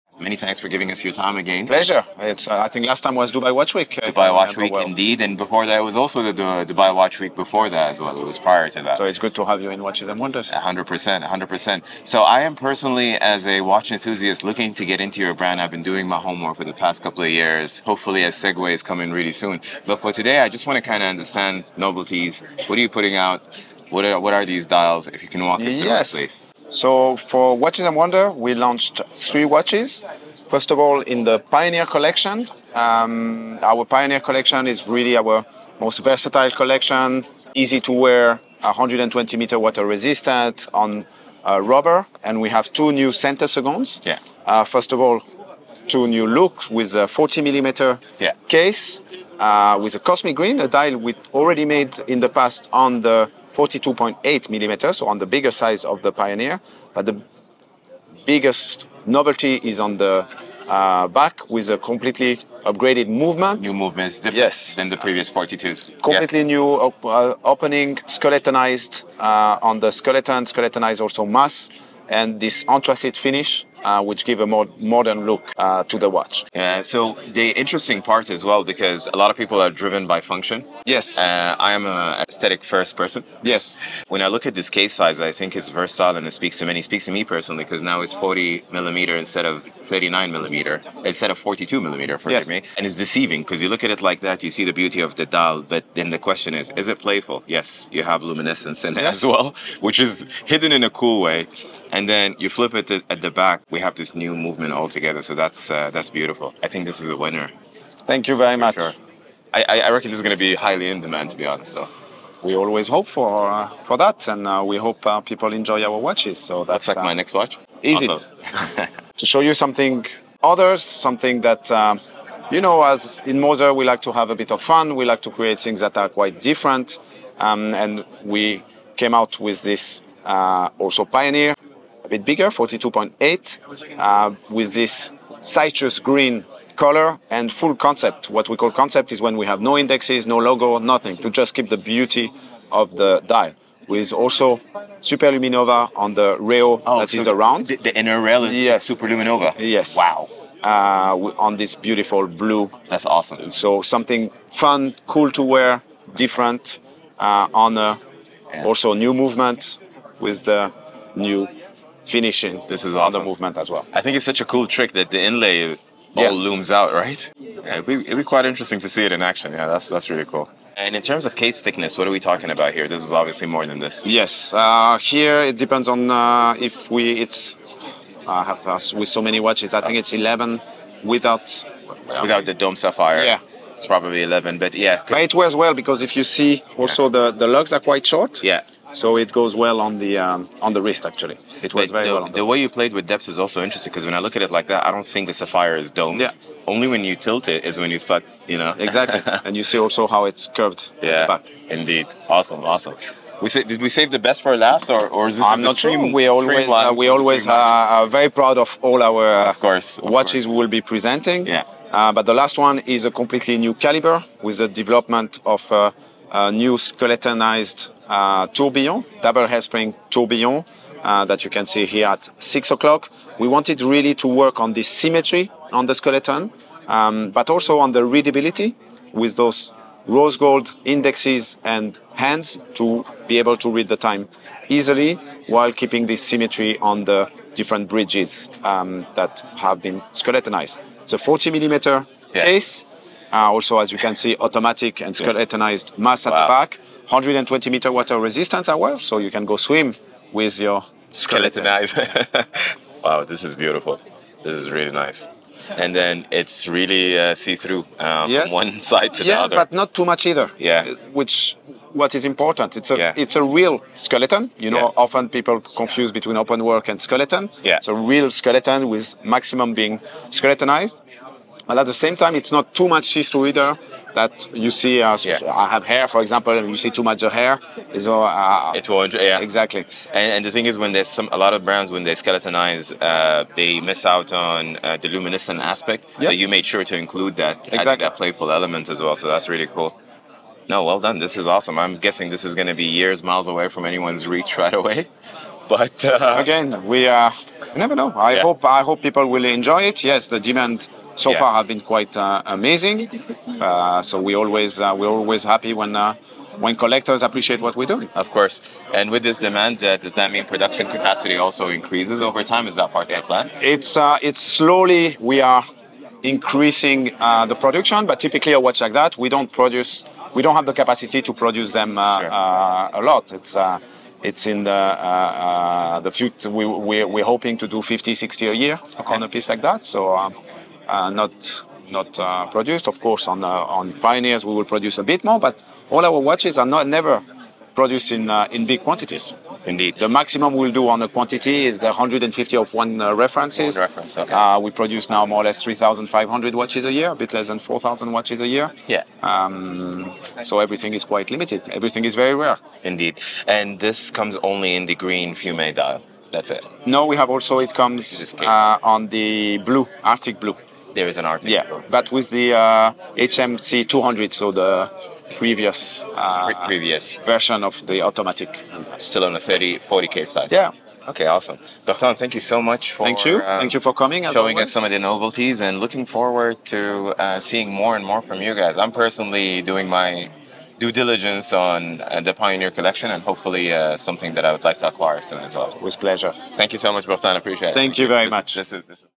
Moser Just Released A Citrus Green Pioneer & Skeleton Streamliner! 🔥
This Citrus Green Pioneer & Streamliner Are Fire! 🔥 H. Moser & Cie 2024 Releases
ALL NEW H. Moser & Cie Pioneer Citrus Green & Streamliner Tourbillon Is Fire!